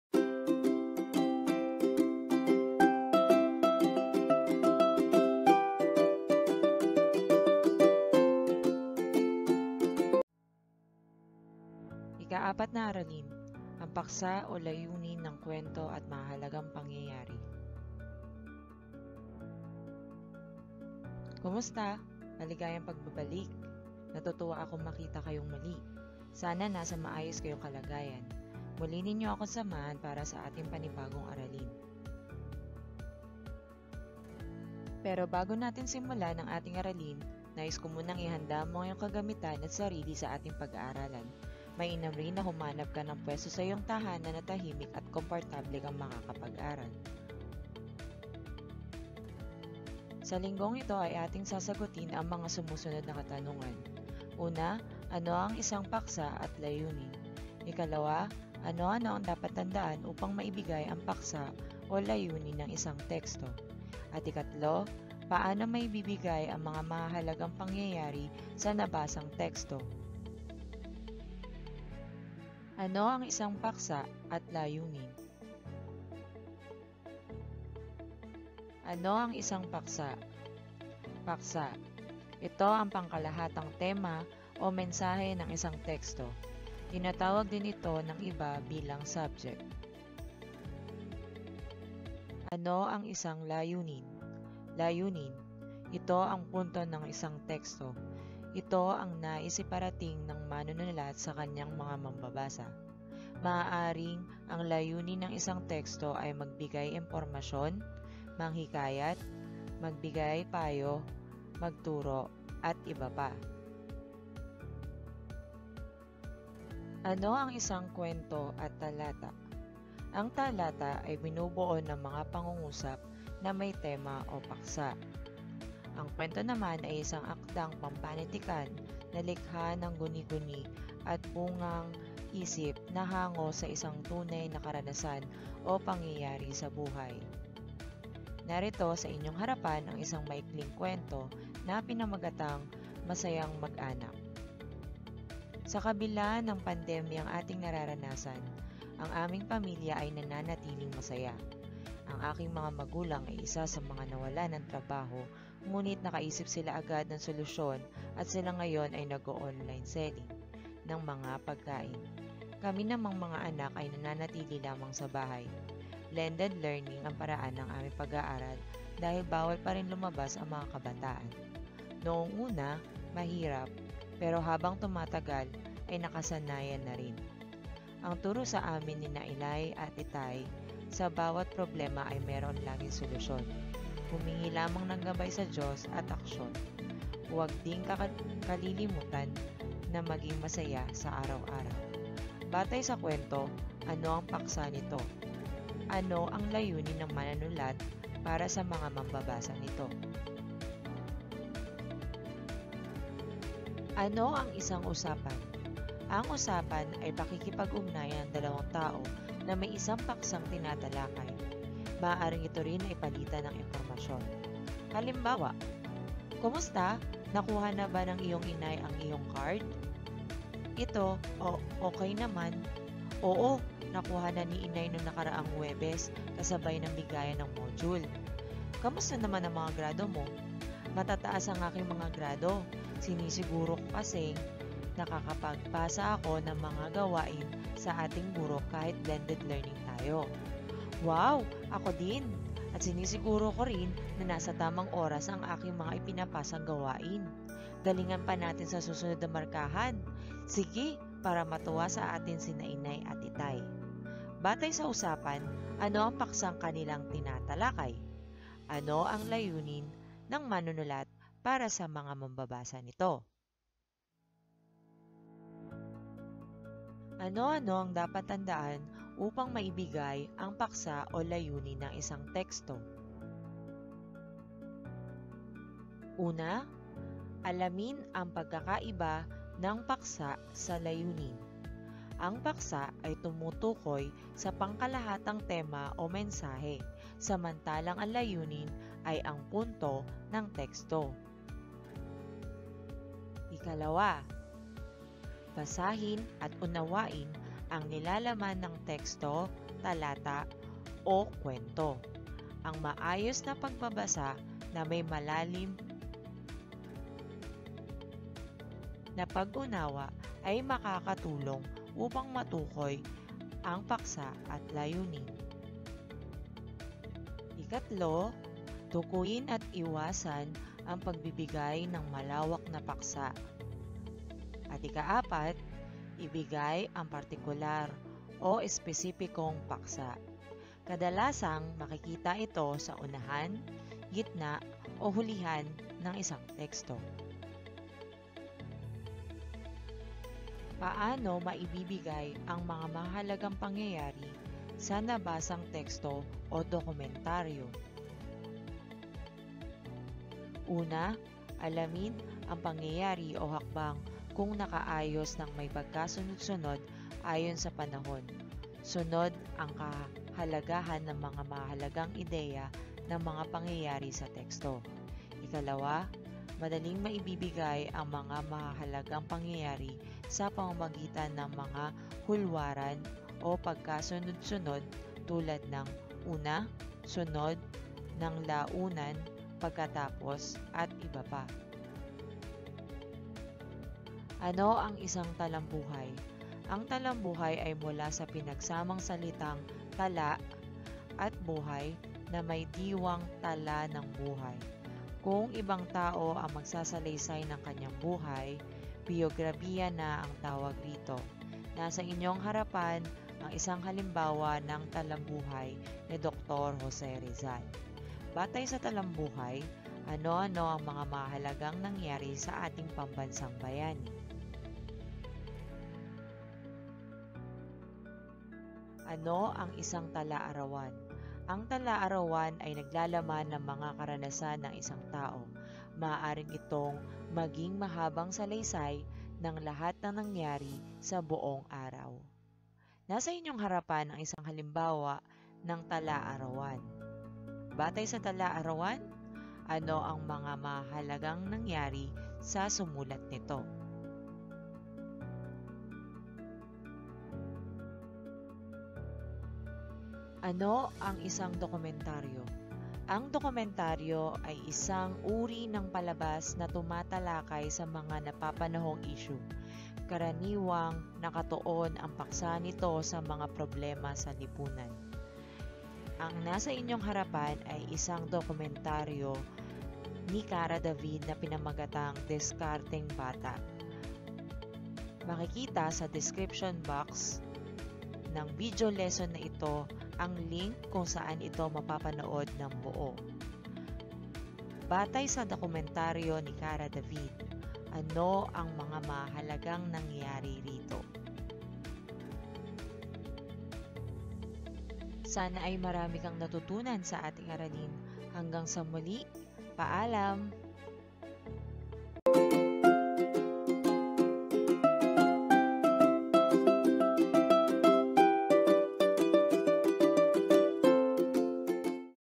Grade 5 Filipino MELC BASED Quarter 2 Aralin 4 Ang Paksa o Layunin ng Kuwento at Pangyayari
Naibibigay ang paksa o layunin ng nabasang kuwento, usapan o talata
F5PD-IIf-I3
Naibibigay ang mahahalagang pangyayari sa nabasang talambuhay,talaarawan, o dokumentaryo
F5PB-IVi-I4

Tinalakay dito kung ano ang layunin at paksa ng nabasang kuwento, usapan o talata. Ibinahagi rin dito kung paano ibabahagi ang mga mahahalagang pangyayari sa nabasang talambuhay, talaarawan o dokumentaryo

Kung nagustuhan mo ang videong ito, i-like, i-share at magsubscribe ka na din sa aking channel.

Maraming Salamat!

Musician:  Free Music by MUSIC4VIDEO